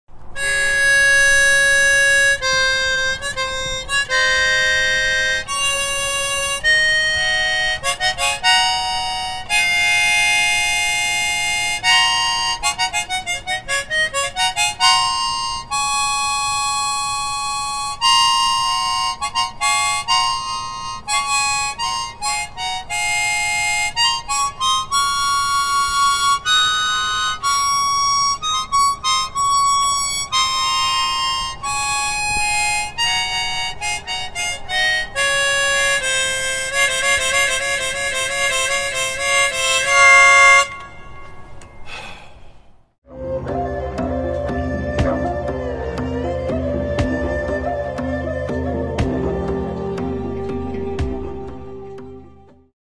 Sadhguru relaxing by finding his way through the harmonica notes | Ride With Safhguru
After a full day's ride, Sadhguru relaxing by finding his way through the harmonica notes.

Music Credits -
Video Clip Credits -